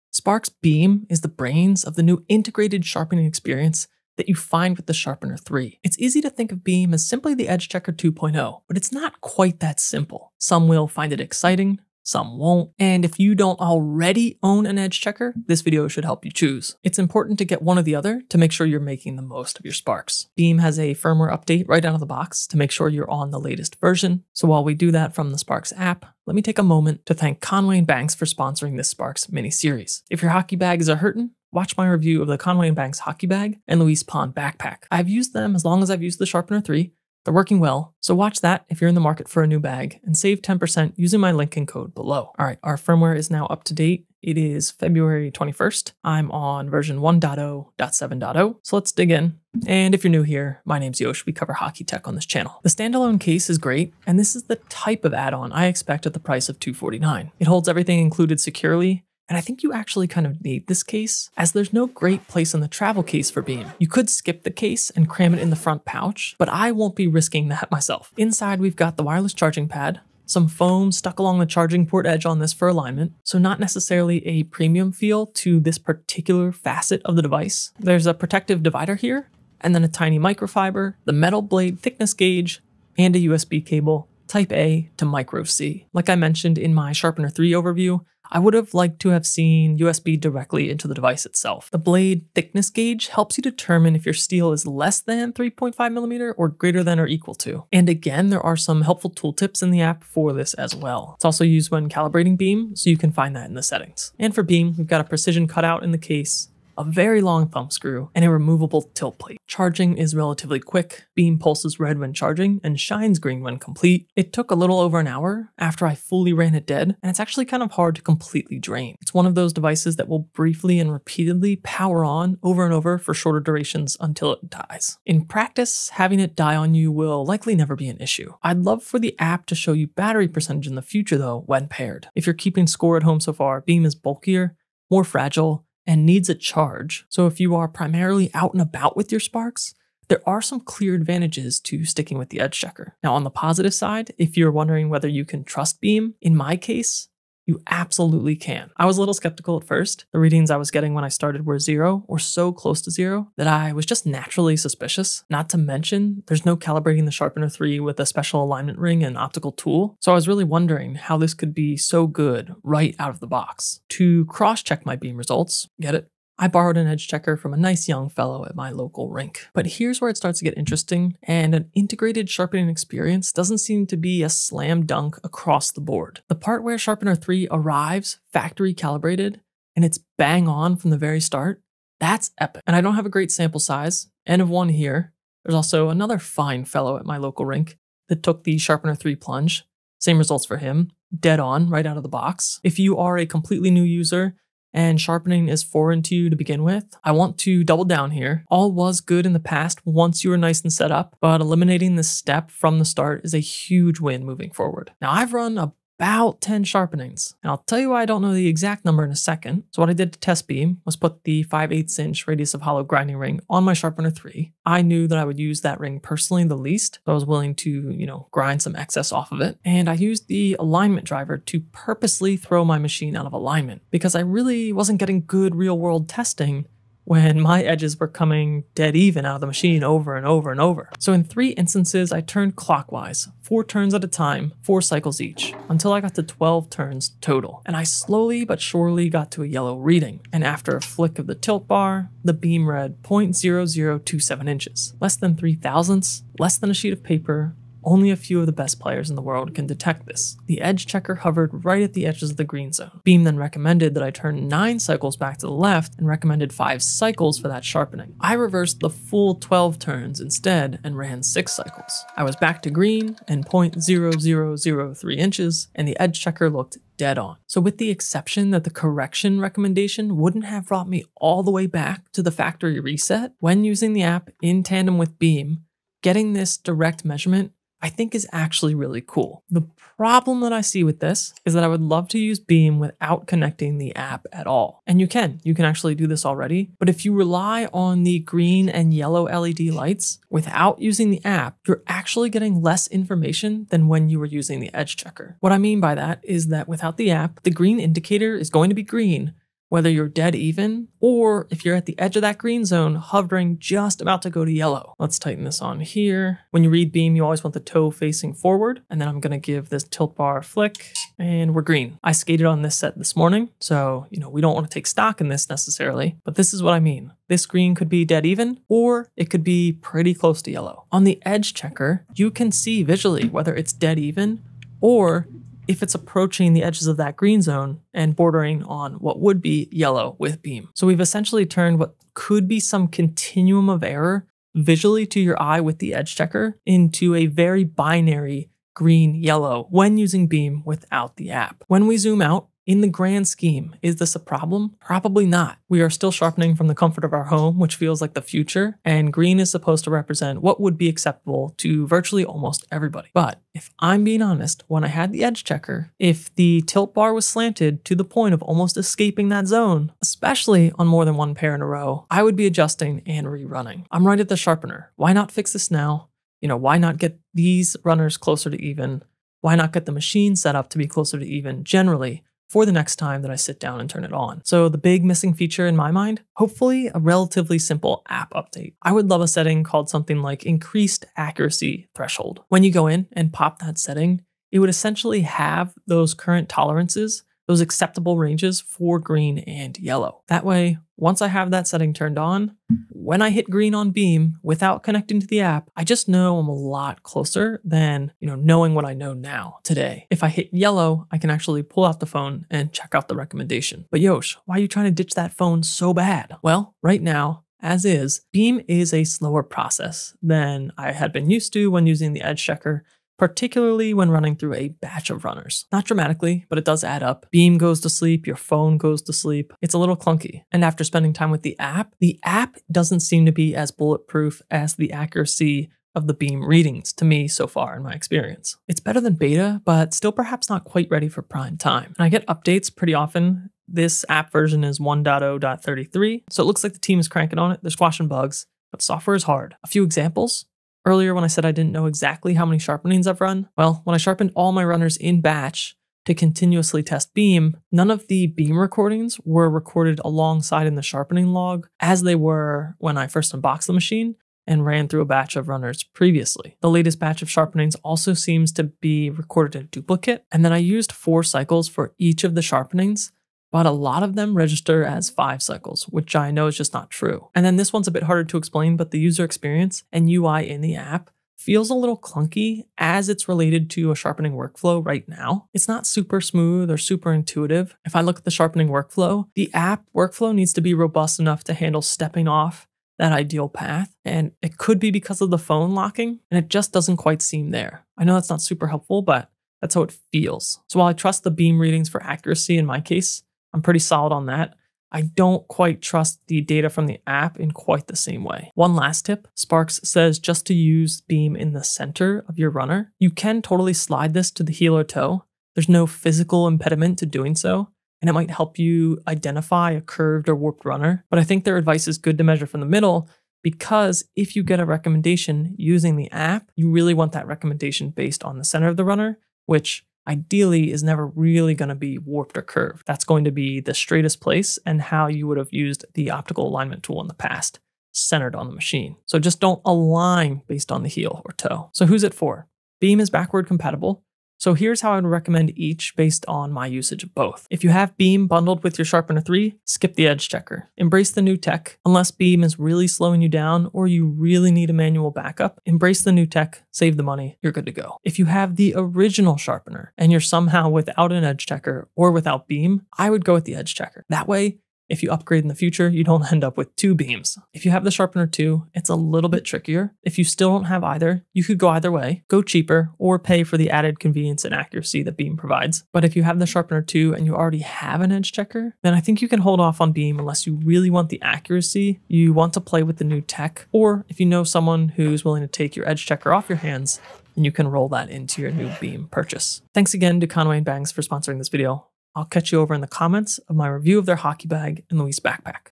Sparx BEAM Review | Missing Feature??
📒 Chapters 👇
0:00 - Sparx BEAM
0:20 - BEAM Firmware Update
0:52 - BEAM Case & Quality
1:31 - Blade Thickness Gauge
1:49 - Charging & Battery
2:23 - Can you trust BEAM?
3:25 - Sparx BEAM vs. The Edge Checker
6:14 - The Missing Feature?
6:47 - The Sparx Hockey App
7:20 - Software Quirks
8:18 - Tips for BEAM
8:52 - My BEAM Recommendation